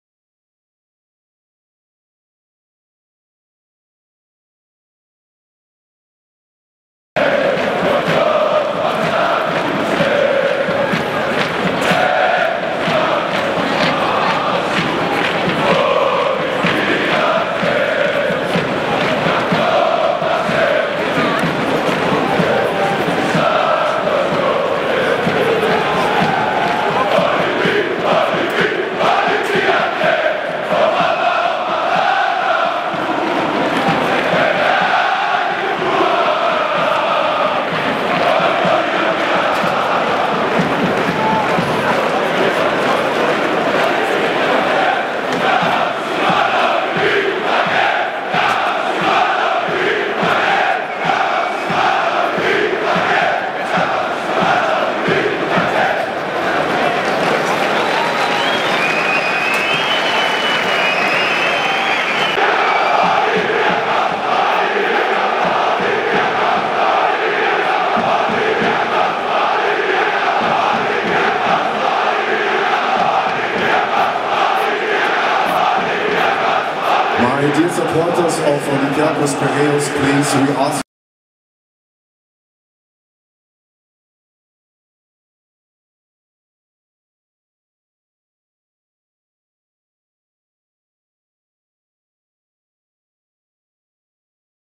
Ο κόσμος του Ολυμπιακού στην Αllianz Arena στο Μπάγερν - Ολυμπιακός (6/11/2019)
Δυναμική η παρουσία των φιλάθλων των Πειραιωτών στο ματς των ομίλων του Τσάμπιονς Λιγκ, δείτε όσα κατέγραψε η κάμερα του a-sports.gr